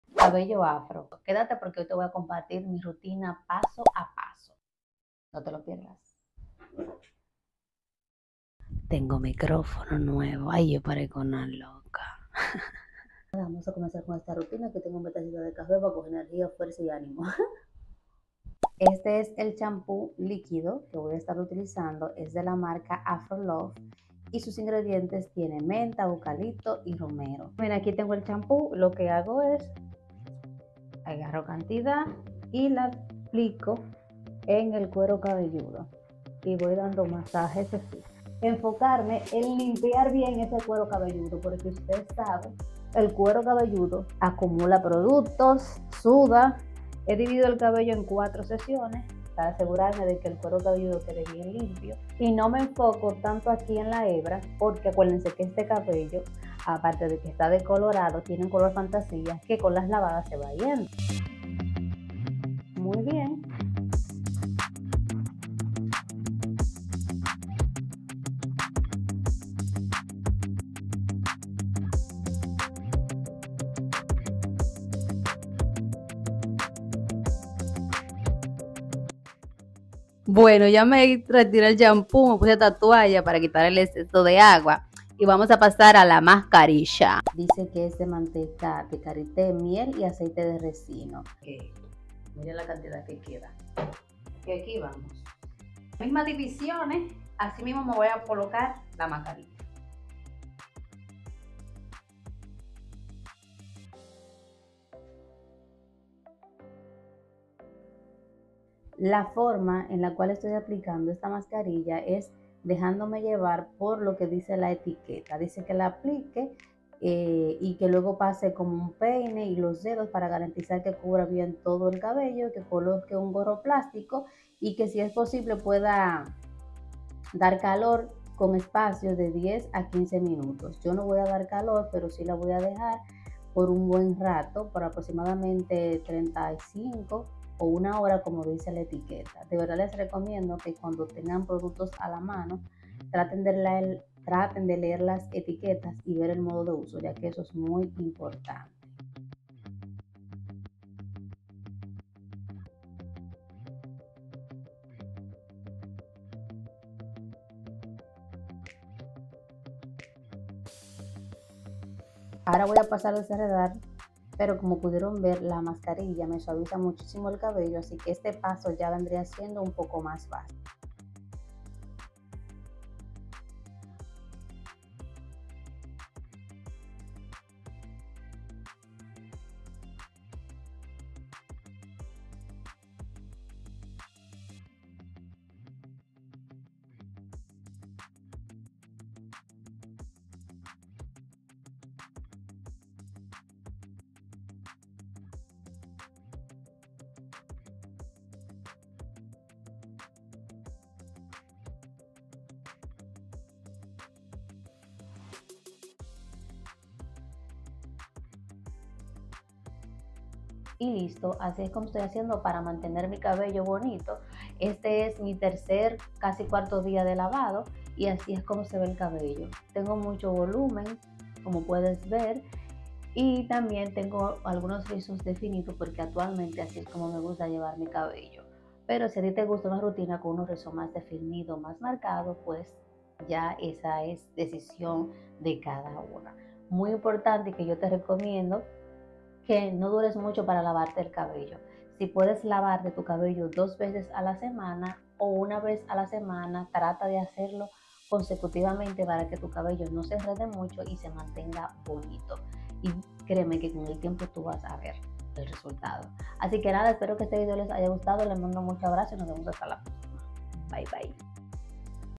NO PUEDO CREER 🔥CABELLO AFRO RUTINA DIA DE LAVADO
CABELLO AFRO RUTINA DIA DE LAVADO
el cabello afro tiene carateristica que es importante saber para asi poder saber cuales son sus necesidades aqui te comparto mi rutina .